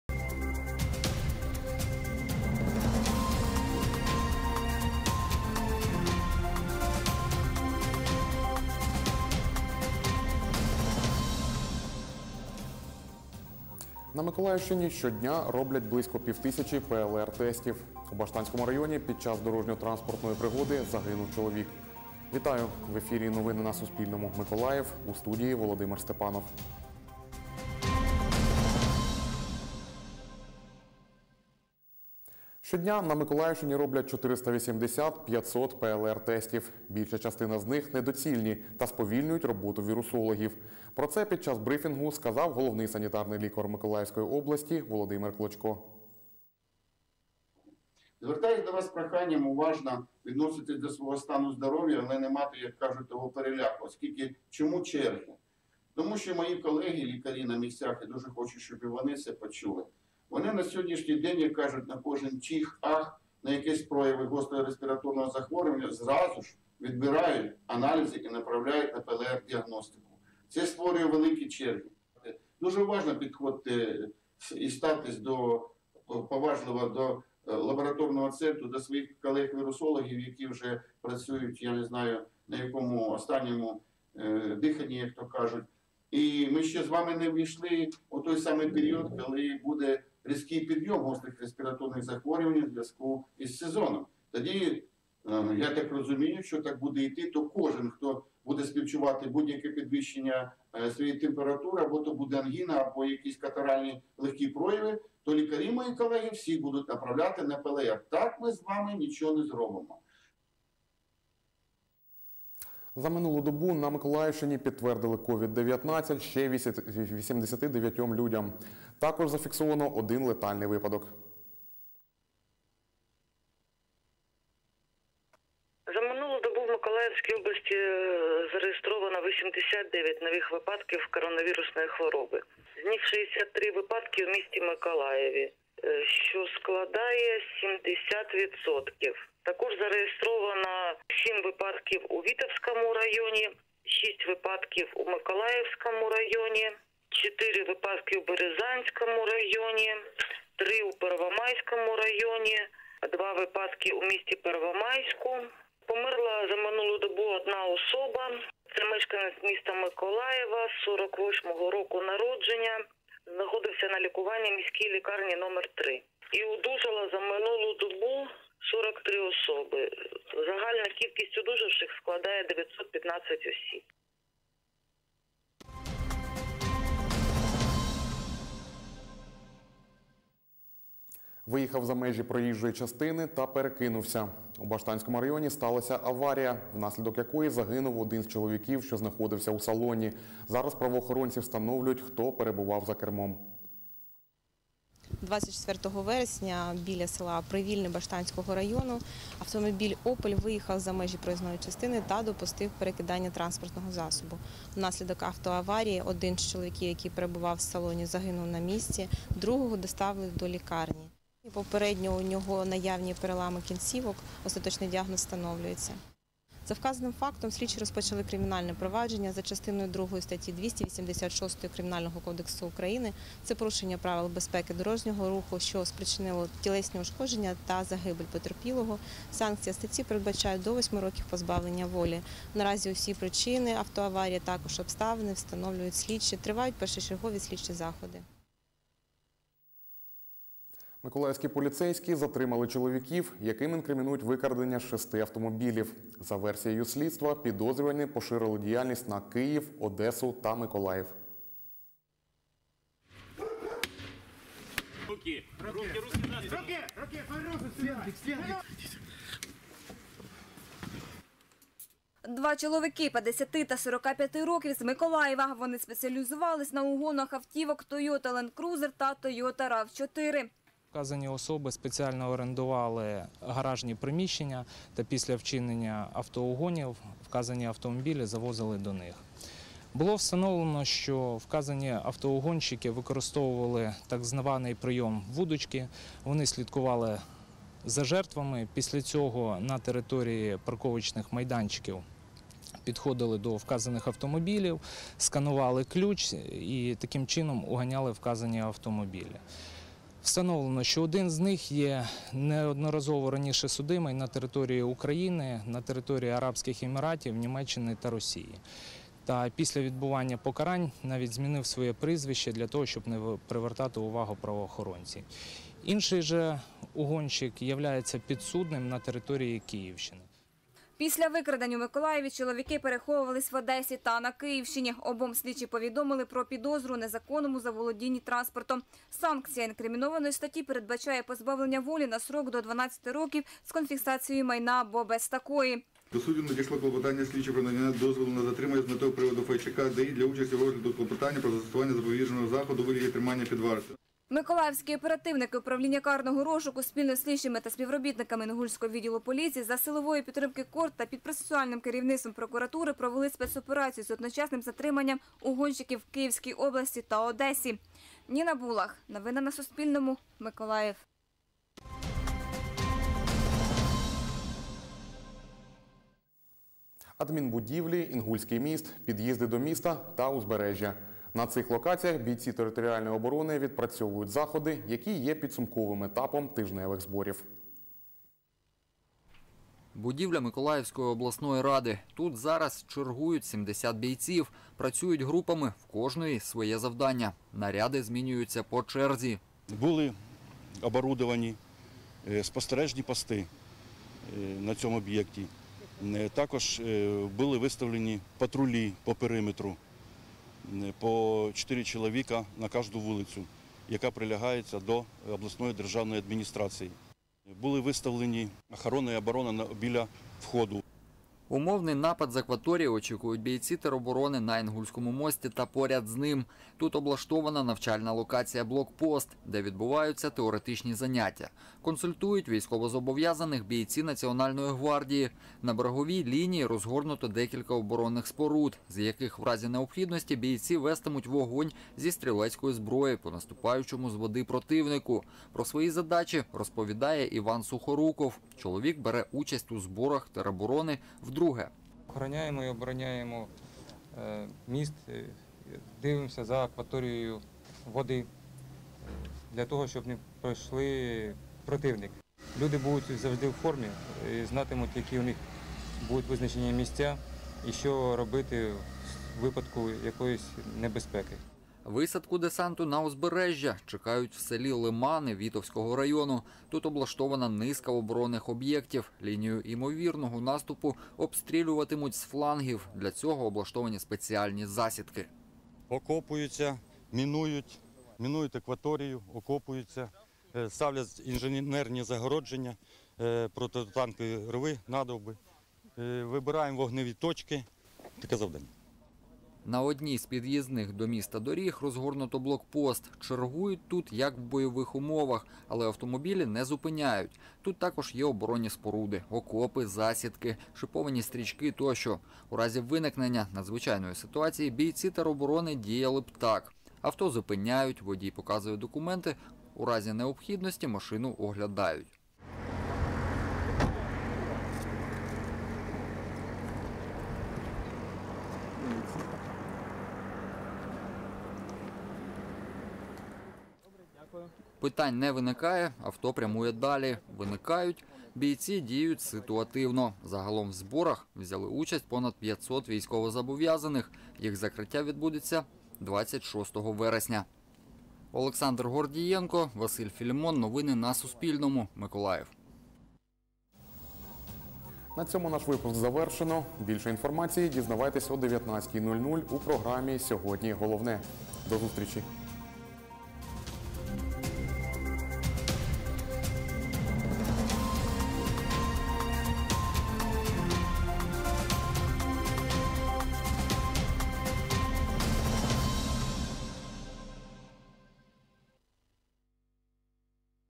24.09.2020. Новини. 17:00
Новини
Інформаційна картина дня (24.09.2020) поглядом кореспондентів телеканалу Суспільне Миколаїв (випуск новин — 17:00):
- Щодня на Миколаївщині роблять 480 – 500 ПЛР-тестів. Більша частина з них – недоцільні та сповільнюють роботу вірусологів.
- За минулу добу на Миколаївщині  підтвердили COVID-19 ще 89-м людям. Також зафіксовано один летальний випадок.
- Виїхав за межі проїжджої частини та перекинувся. У Баштанському районі сталася аварія, внаслідок якої загинув один з чоловіків, що знаходився у салоні. Правоохоронці встановлюють, хто перебував за кермом.
- Миколаївські поліцейські затримали чоловіків, яким інкримінують викрадення 6-ти автомобілів. За версією слідства, підозрювані поширили діяльність на Київ, Одесу та Миколаїв.
- Адмінбудівлі, Інгульський міст, під’їзди до міста та узбережжя. На цих локаціях бійці територіальної оборони відпрацьовують заходи, які є підсумковим етапом тижневих зборів.